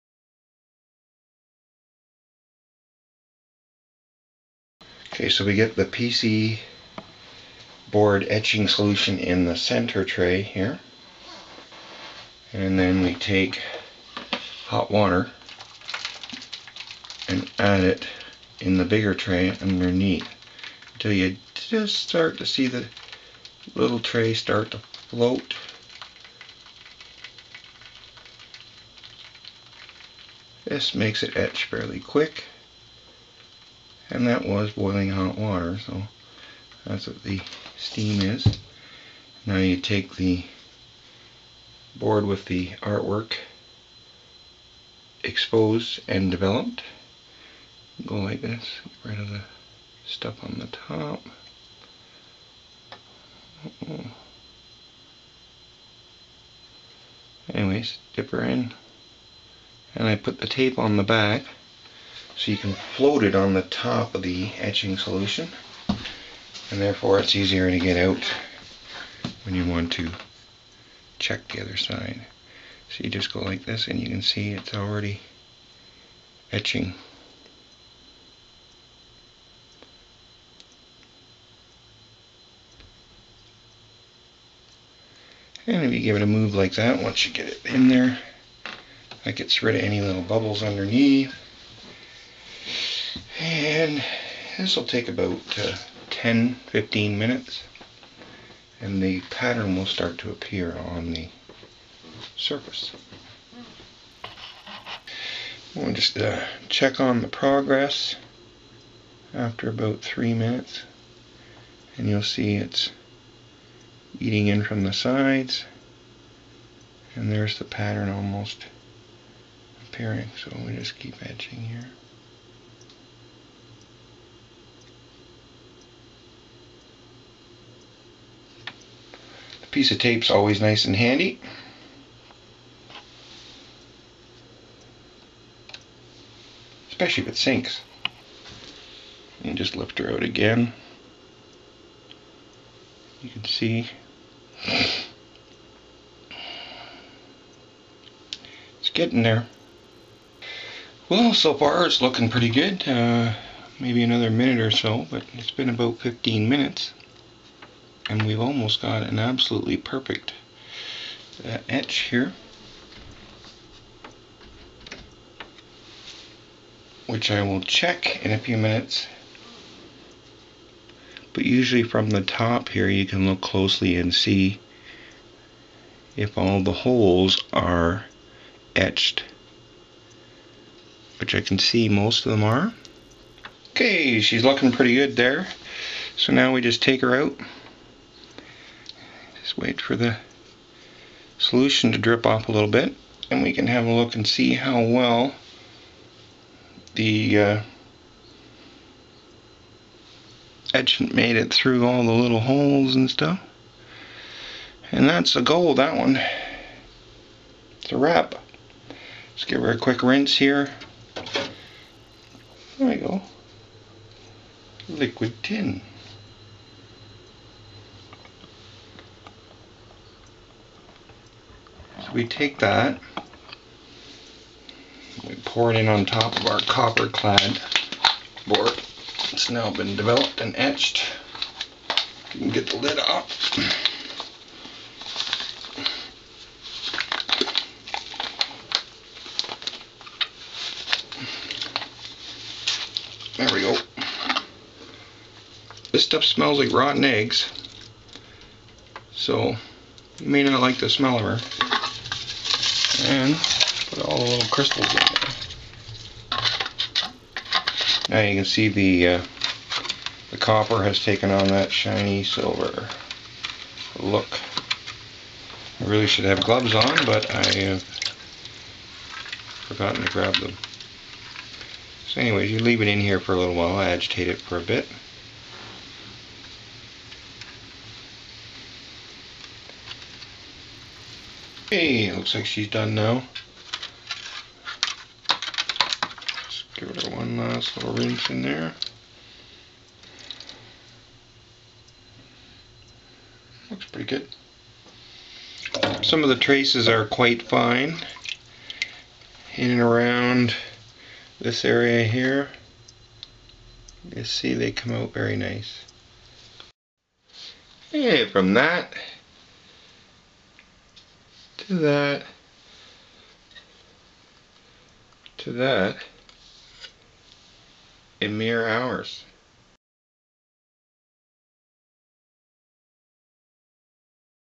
Making a PCB-Printed Circuit Board-DIY-PART II-Etching & Liquid Tin
Step by step process for making Printed Circuit Board (PCB) from a positive artwork. Exposed Copper Clad board is etched in etchant and then cleaned. Finally Liquid Tin plating is applied to copper traces.